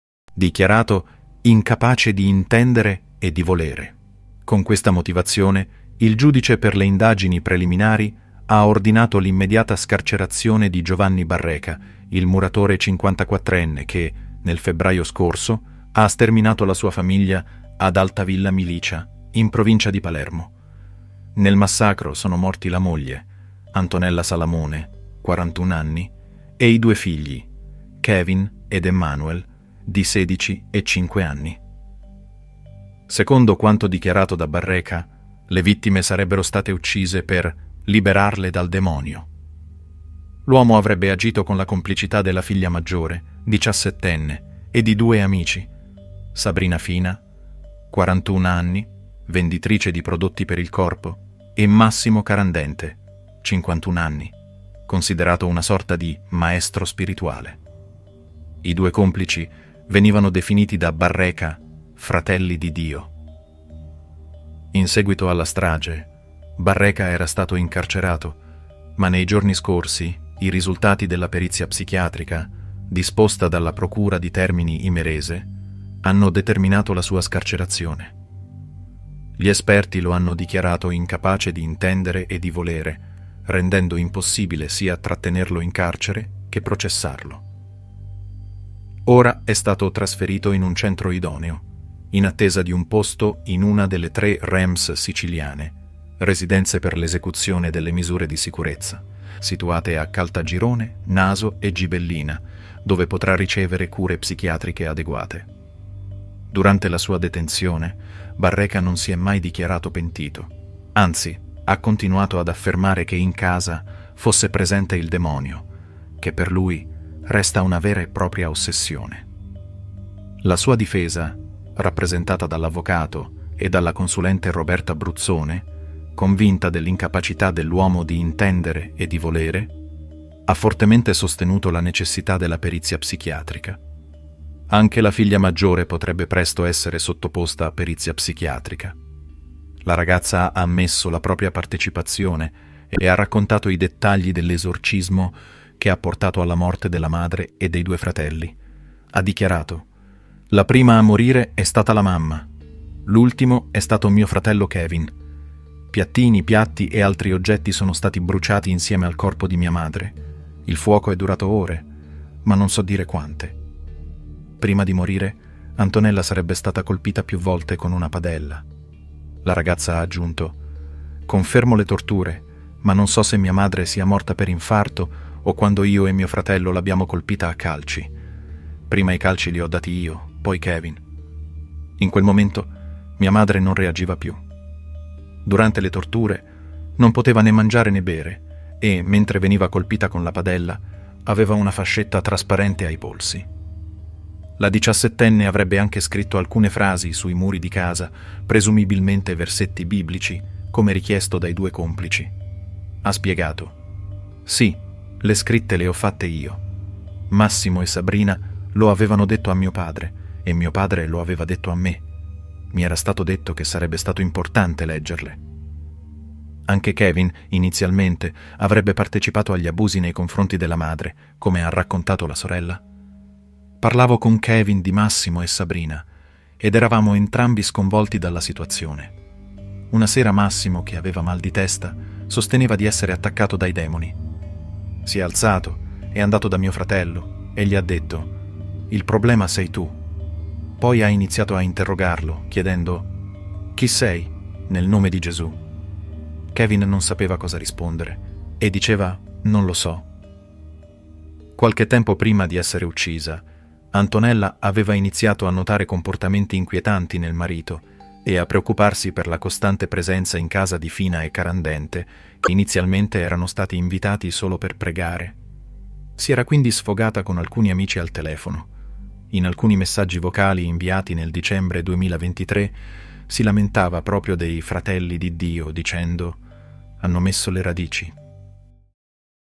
Niente carcere per lui - Giovanni Barreca
Giovanni Barreca era stato arrestato a febbraio per aver sterminato la famiglia.